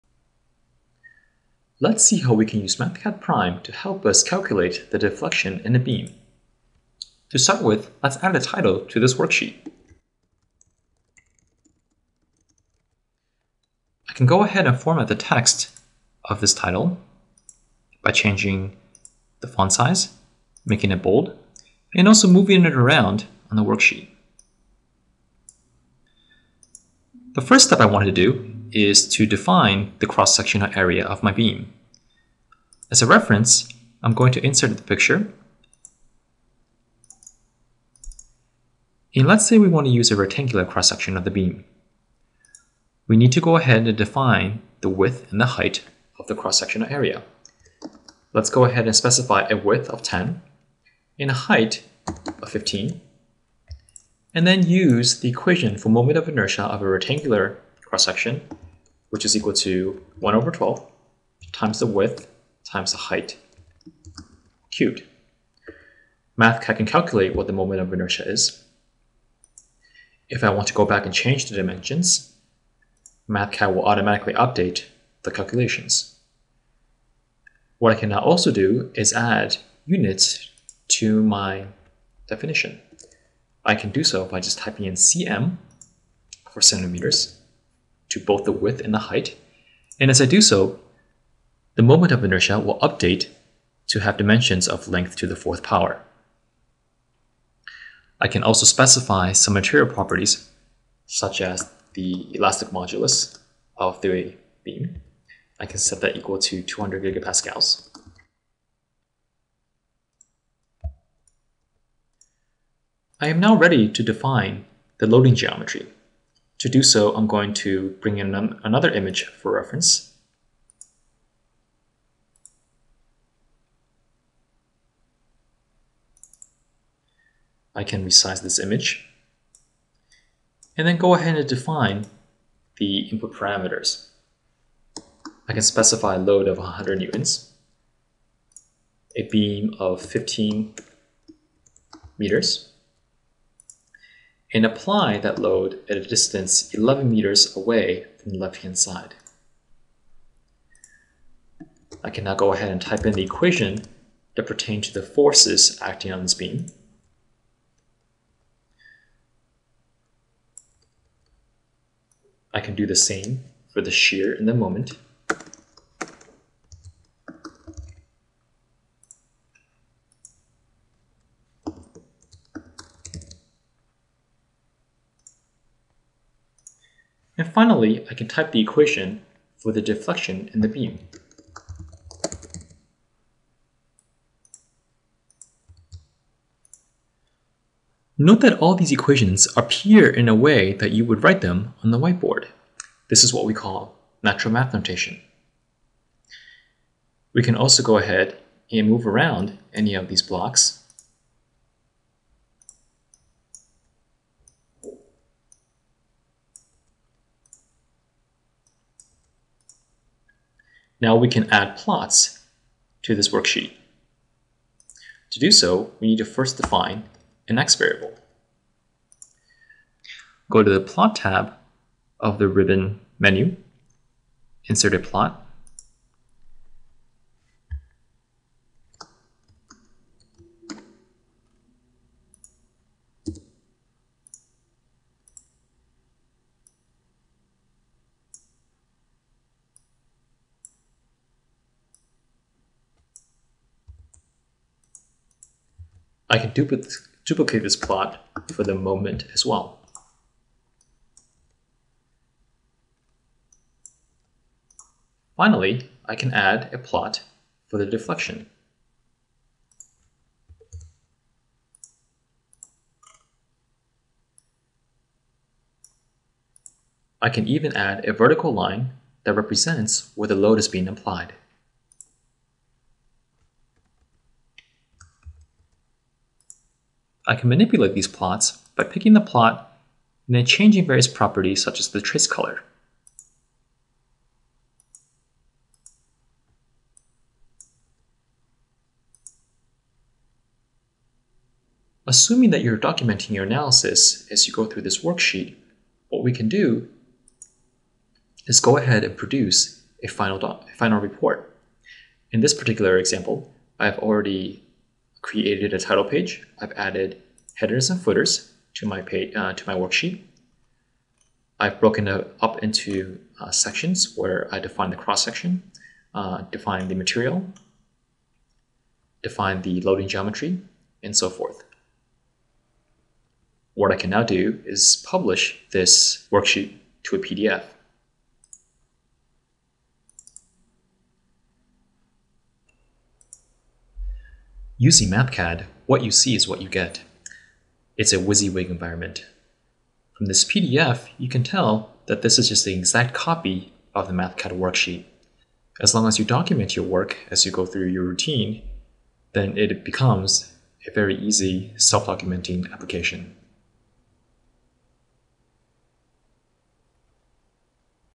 Mathcad 101 Demo -- Calculating the Deflection of a Beam
Mathcad Prime 1.0 possesses outstanding engineering and document handling capabilities. In this video Roger Yeh demonstrates how to use Prime's symbolic, unit-handling, graphical, and document formatting capabilities to calculate the deflection of a beam under load and produce an engineering report documenting the calculations as intellectual property.